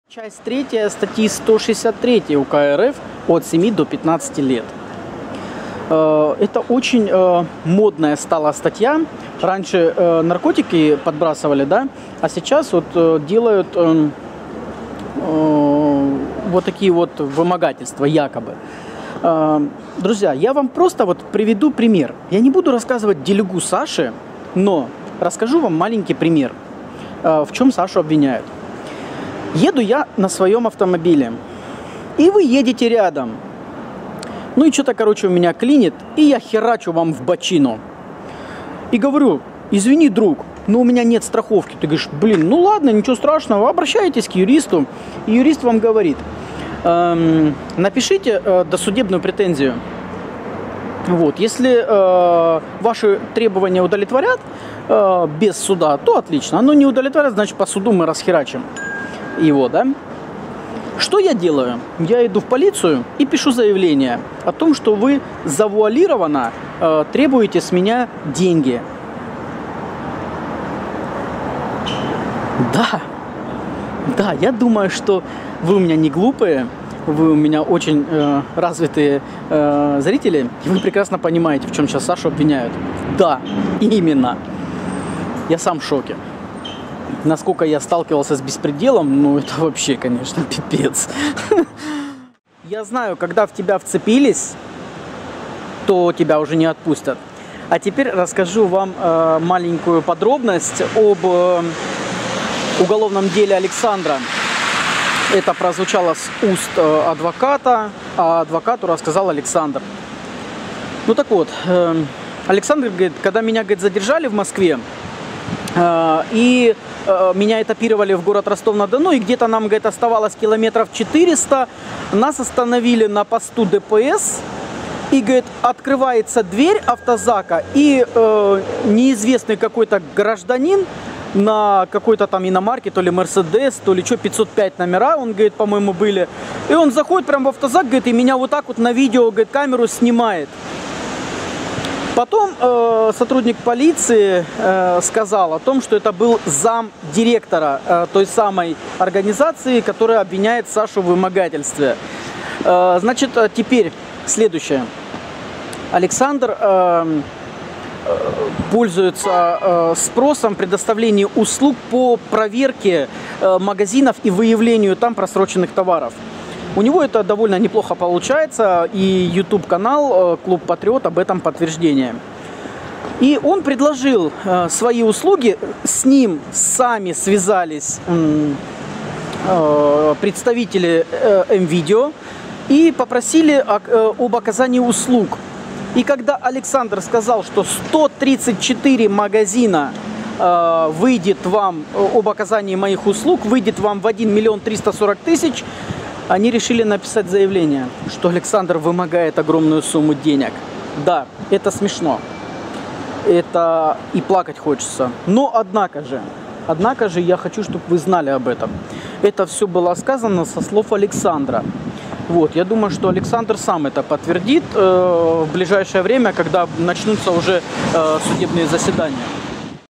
САН САНЫЧ "КЛУБ ПАТРИОТ" - ЗА РЕШЁТКОЙ! / ДЕТАЛИ ДЕЛА от Гаспара Авакяна
САН САНЫЧ "КЛУБ ПАТРИОТ" - ЗА РЕШЁТКОЙ!

Сан Саныча "Клуб патриот" задержали ПО ПОДОЗРЕНИЮ в "вымогательстве". Для того, чтобы оставить человека в Следственном Изоляторе должны быть ОСНОВАНИЯ - собранный материал дела с имеющимися основаниями оставить человека под стражей на время следствия (п.1 ч.2 ст.29 УПК РФ; ст.108 УПК РФ)

По предварительной информации, судить Маркина А.А. будут по статье 163 УК РФ, ч.3, п."Б", "Вымогательство", грозит ему от семи до пятнадцати лет. За ходом дела можно следить по ссылке

Как считаете, виновен Сан Саныч или НЕТ? А может быть дело "заказное"?

Моё личное мнение.
Сан Саныч предложил свои услуги по выявлению просрочки и прочих замечаний в крупной торговой сети - в этом нет ничего незаконного, так как гражданское законодательство позволяет заключать любые договора, которые прямо не запрещены законом.
Отмечу, что руководители магазинов очень часто заявляет, что у них не хватает работников и якобы из-за этого они не справляются со своими обязанностями. Знаю точно, что эта функция может быть передана и передаётся на аутсорсинг.
В ДИКСИ так, например, было, когда мы в течении недели каждый день проверяли один и тот же магазин (посмотрите видео посмотрите с 6:00 ) пока они не исправились... В том магазине со стороны директора Екатерины была попытка скомпрометировать нас, т.е. провокация путём клеветы о том, что мы вымогаем у неё что-то... (посмотрите

Так вот, полагаю, что у Сан Саныча могли вытянуть фразу о том, что если с ним не заключат указанный выше договор (речь идёт о большой денежной сумме), то будут "последствия"... Например, он у них будет выявлять просрочку с публикацией её результатов в Ютубе и будет подавать заявления в правоохранительные органы и т.п.
Всё это конечно "за уши притянуто", но для того чтобы процессуально прессовать неугодных (а это можно делать годами) этого достаточно!

Надеюсь у Сан Саныча хватит сил, времени и средств чтобы доказать свою невиновность и наказать тех кто его заказал/подставил/закрыл. На 99% уверен, что его дело заказное - исхожу из своего личного опыта.